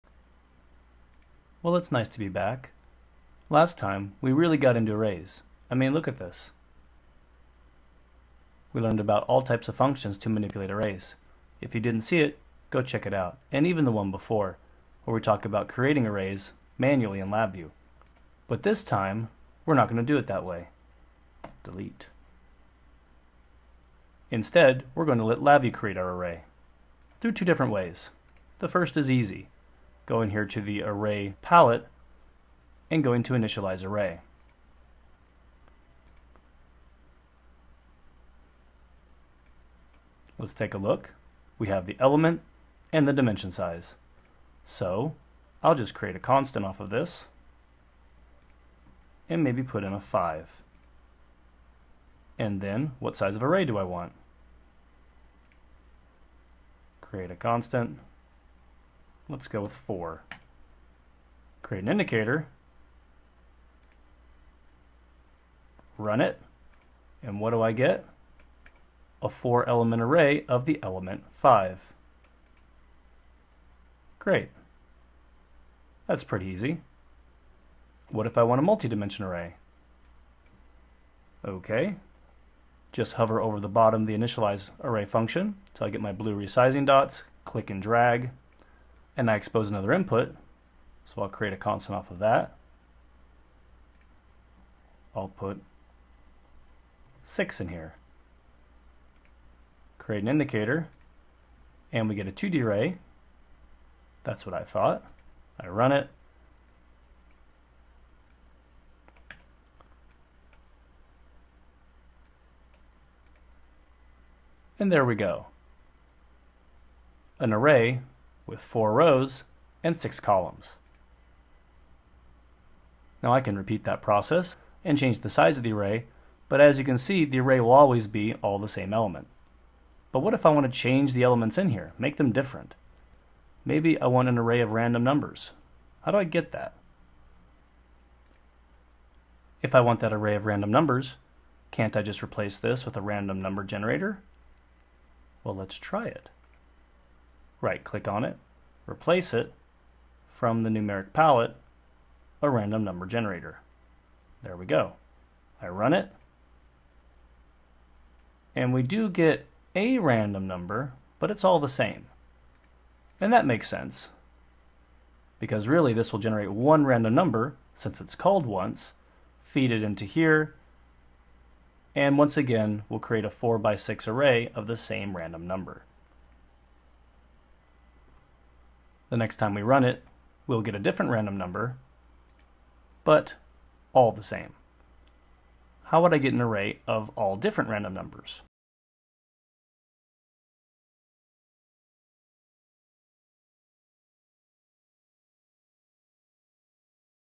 VI High 28 - How to Use the Initialize Array Function in LabVIEW
This episode is part of a VI High series focusing on arrays that runs from VI High 24-32.

This time around we'll look at building an array with the Initialize Array function and learn why it won't be sufficient to build an array of random numbers.

Experience level: Basic